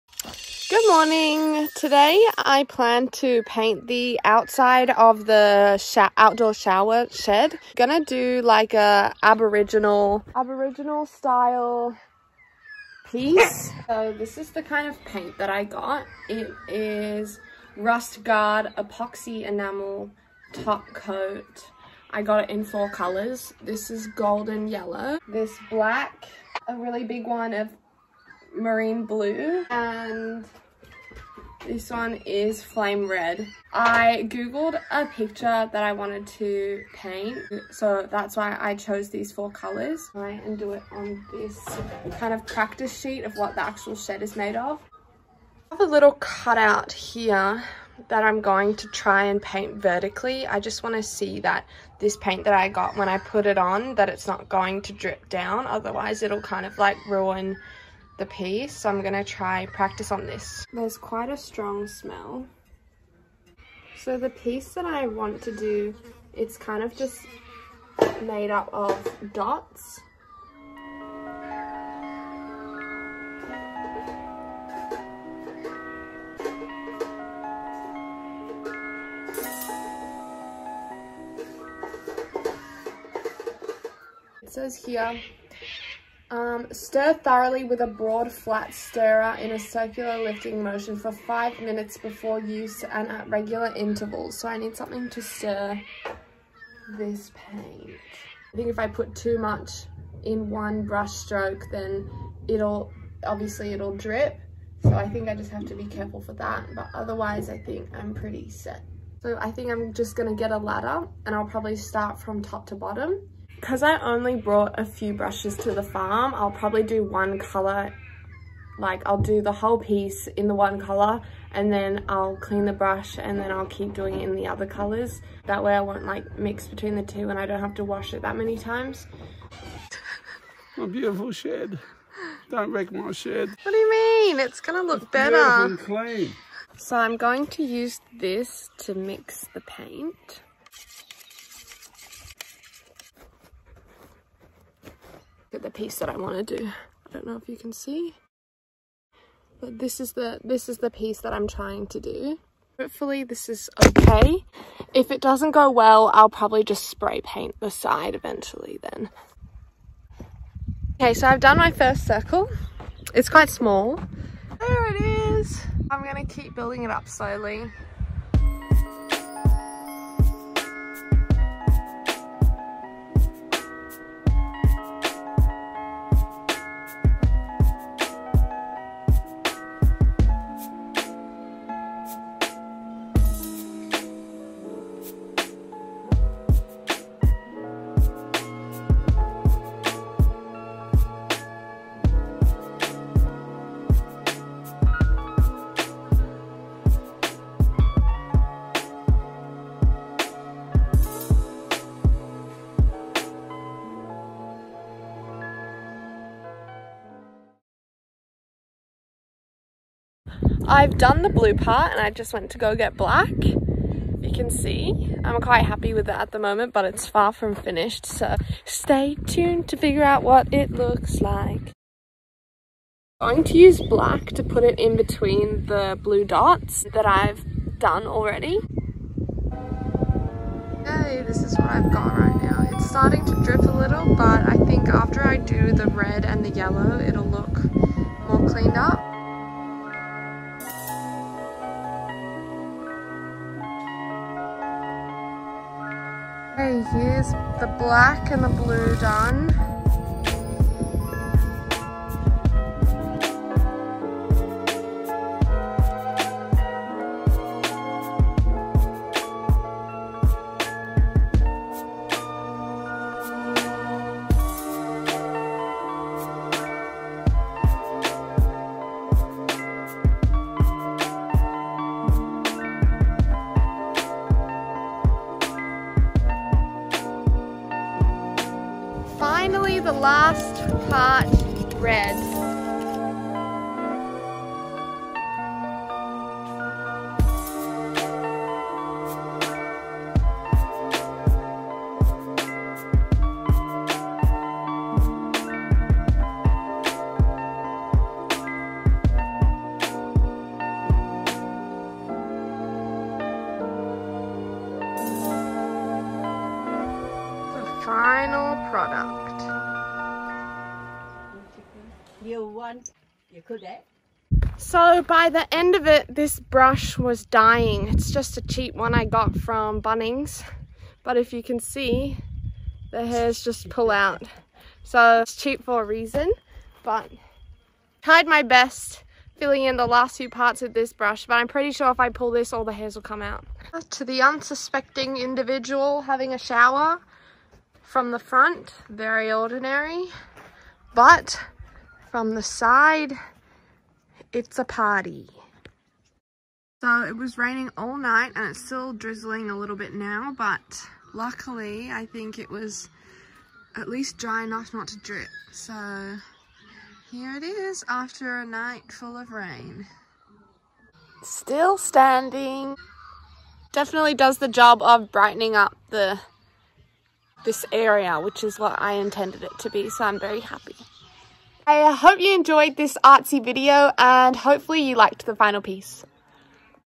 PAINTING an aboriginal mural
A short but colourful aboriginal mural to spice up the outdoor shower shed!